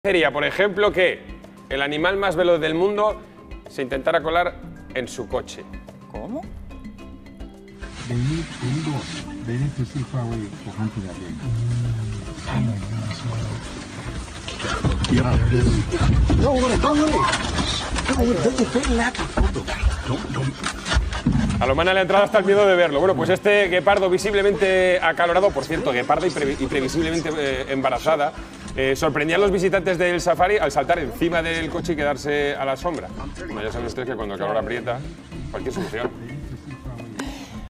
Un guepardo busca refugio del sofocante calor en el jeep de unos turistas
¿Qué te parecería si el animal más veloz del mundo se intentase colar en tu coche?

Es lo que les ha pasado a los visitantes de este safari cuando una hembra guepardo, visiblemente preñada y acalorada, ha encontrado la sombra del techo del jeep de estos turistas Toda la info de este y otros temas de Juntos en ▶️